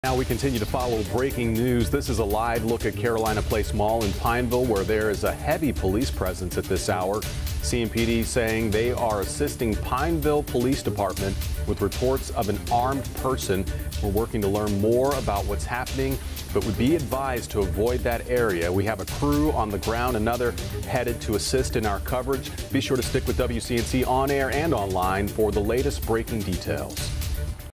Heavy police presence at Carolina Place Mall in Pineville, NC
Emergency personnel are evacuating and searching Carolina Place Mall in Pineville following unconfirmed reports of a person with a gun.

As of 5:30 p.m., emergency officials were investigating a reported shooting. Initial searches of the property discussed over emergency radio communications indicated there were no immediate injuries discovered.

Police were continuing to search the building for any threats.

Little additional information was immediately known but WCNC Charlotte has a crew at the scene and will be providing updates on-air and online as information becomes available.

Shoppers evacuated from the mall needing to reunite with family and friends, are advised to go to the Sam's Club located across the parking lot to the south of the main mall building.